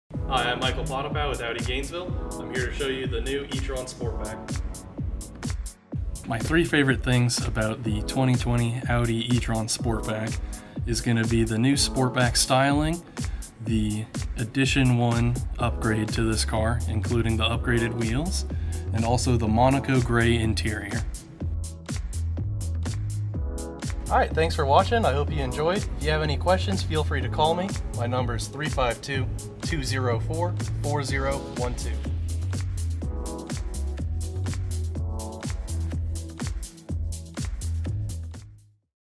Quick Look Into This 2020 Audi Etron Sportback
Michael Potapow is one of our Audi Gainesville Brand Specialists and here he gives us a quick look at this 2020 Audi Etron Sportback available at our Audi Gainesville dealership located in  1920 N Main St, Gainesville, FL 32609.